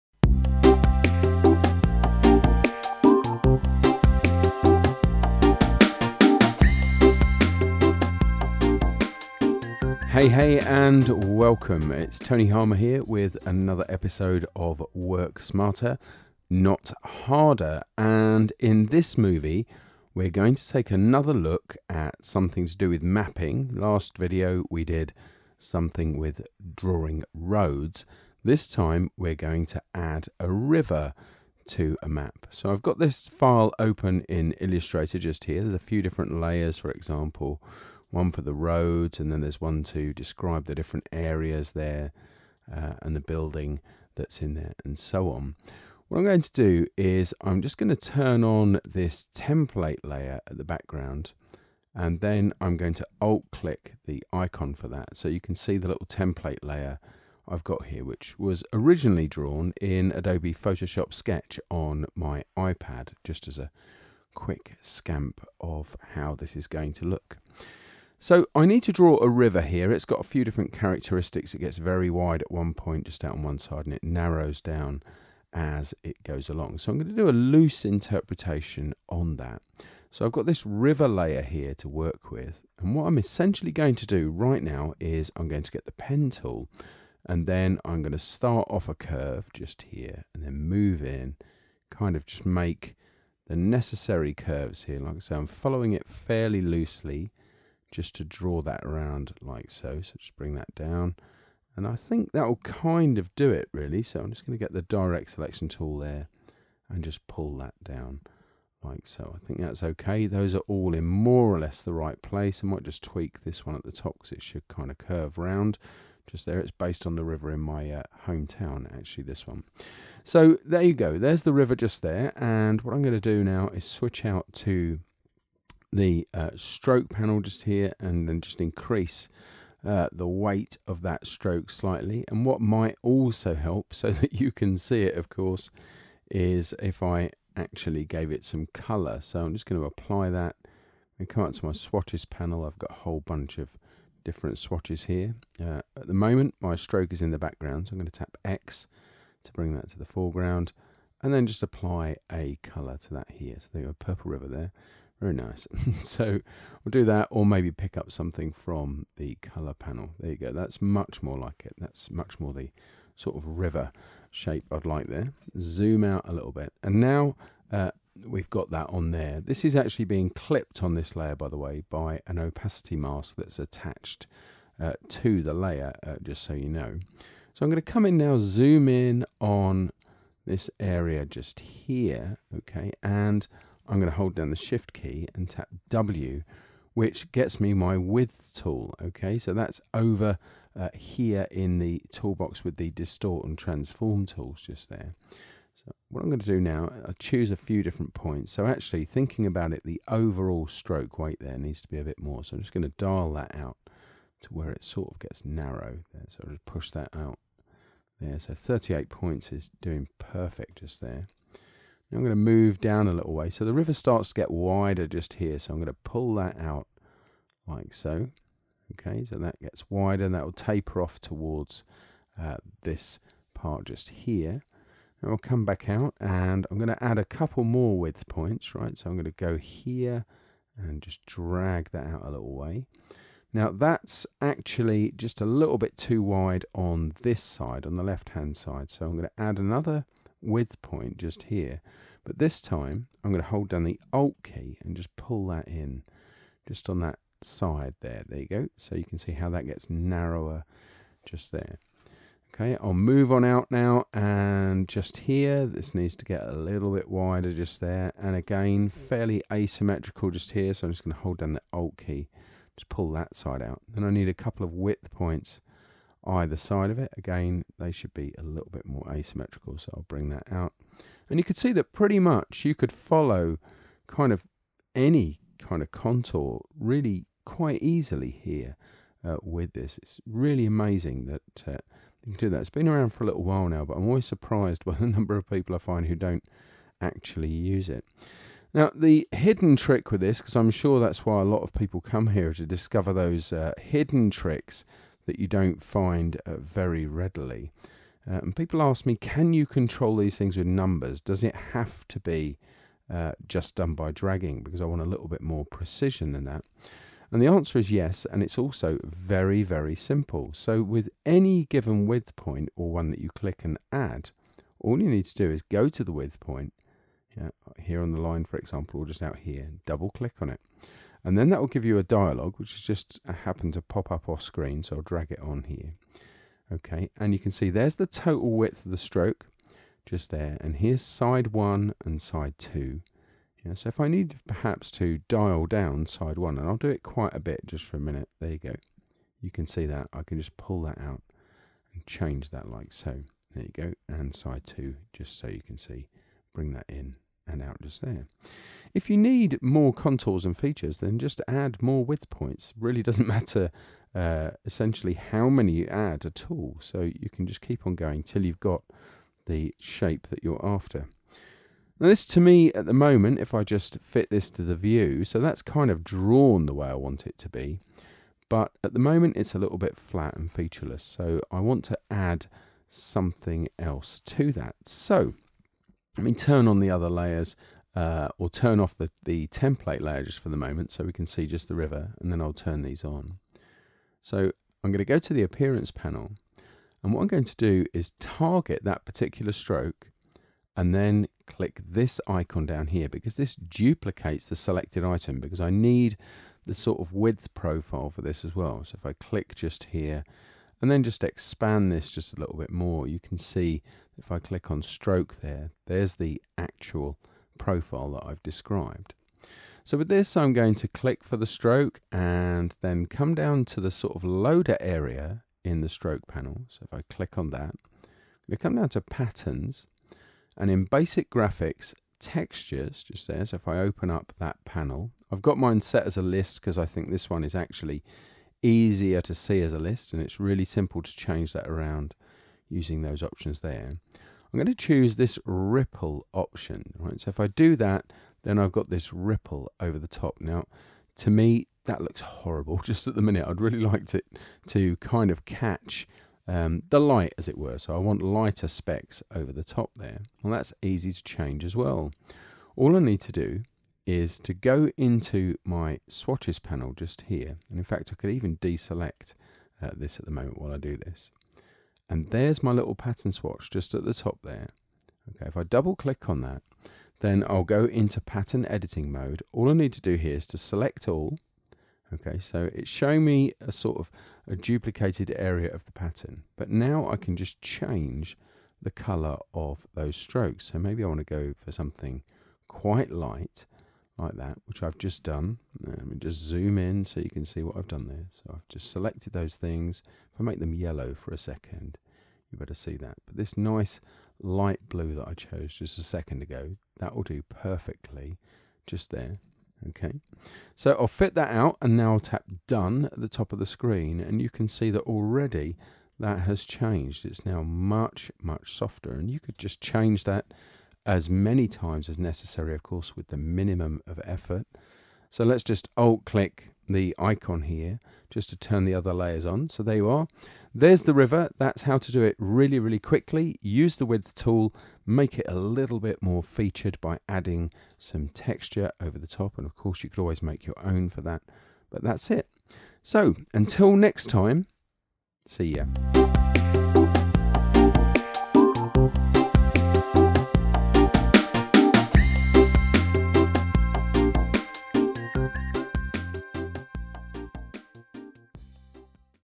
WSNH9: Drawing Rivers on Simple Maps in Illustrator
Following on from WSNH8, this movie looks at adding rivers, quicly and easily.